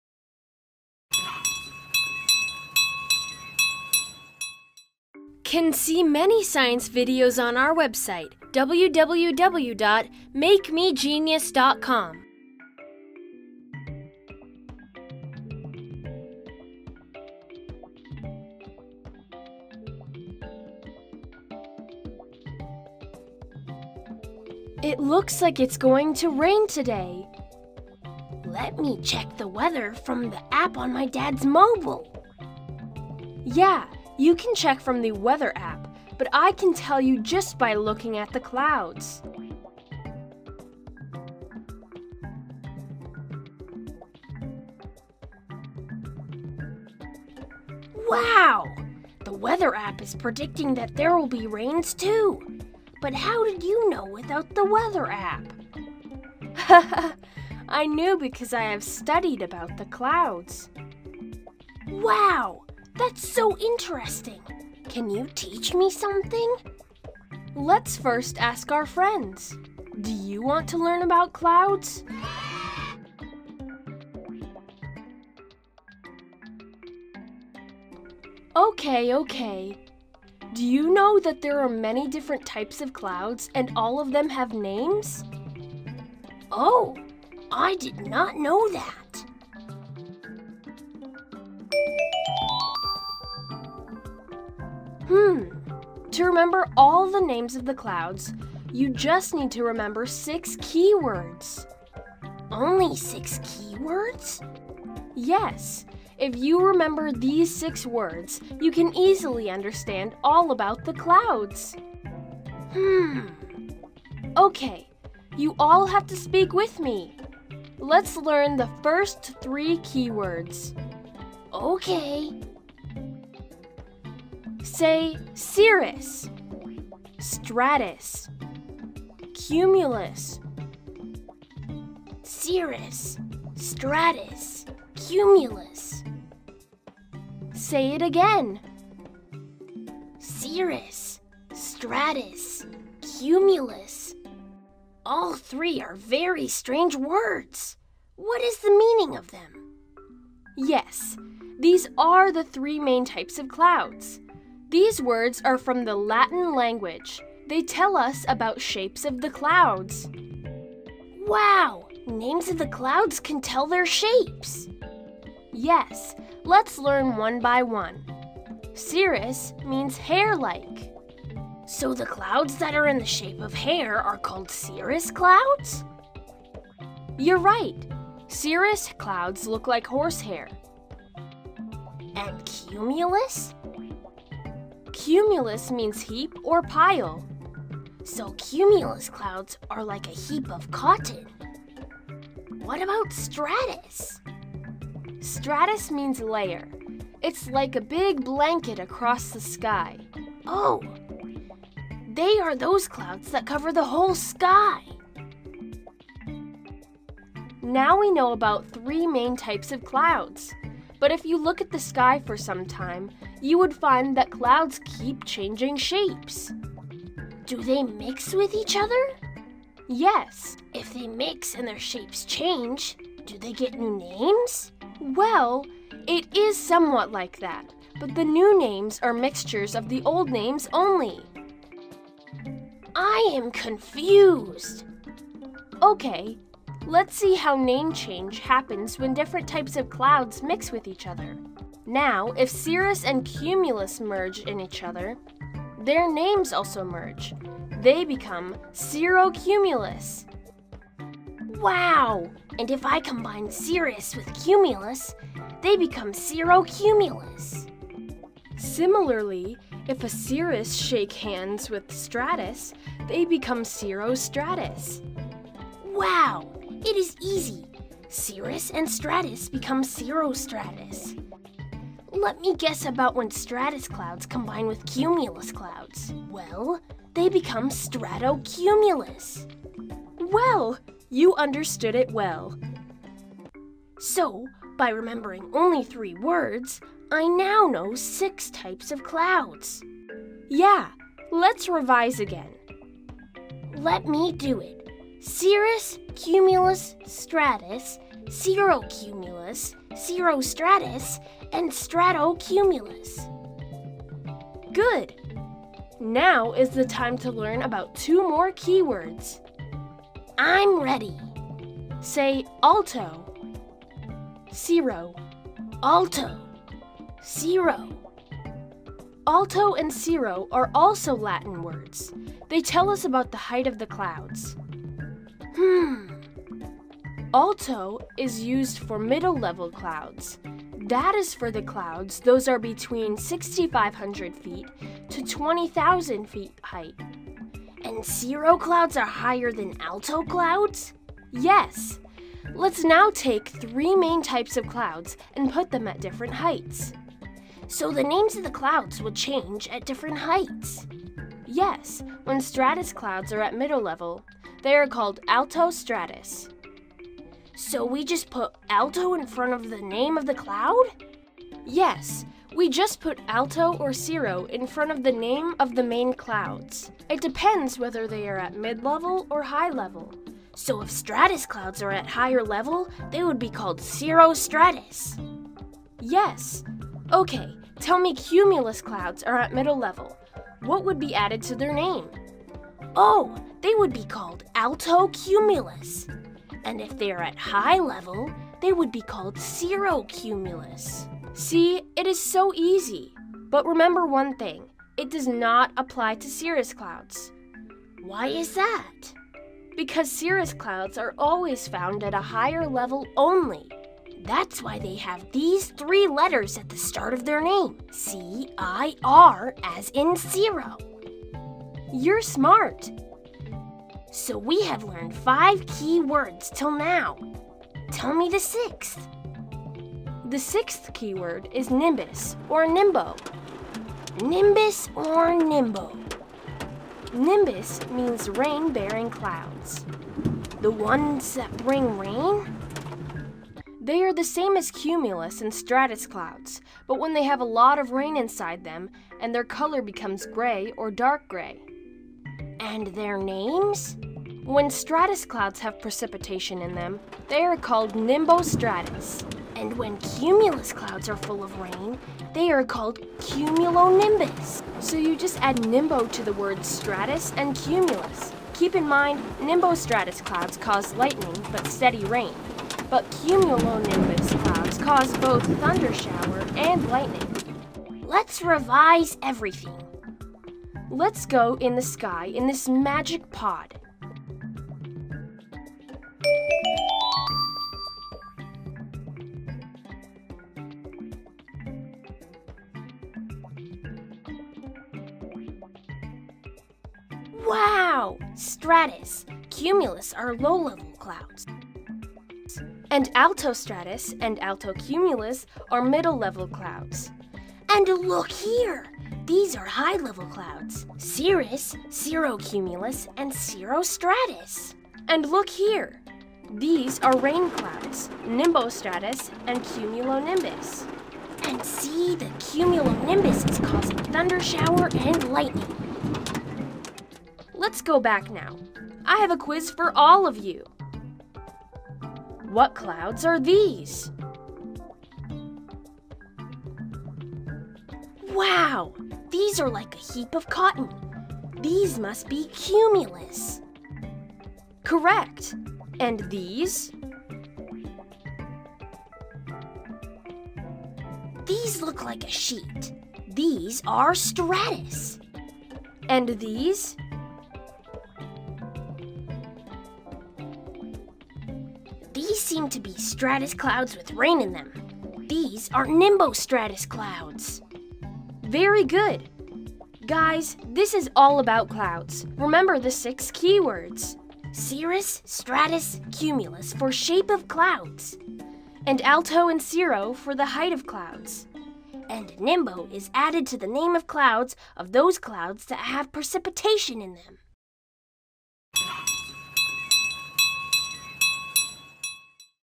Cloud Types -for kids.
Clouds form in three basic patterns:

1.Cirrus
2. Stratus
3. Cumulus

When clouds combine with each -other , a number of different cloud combinations are created.

The clouds that bring rains : The Latin word for shower is nimbus. So the technical term for describing the cloud associated with thunderstorms is cumulonimbus.

Clouds are also distinguished by the heights above ground level at which they form.
1. High Level
2. Middle  level (Alto)
3. Low level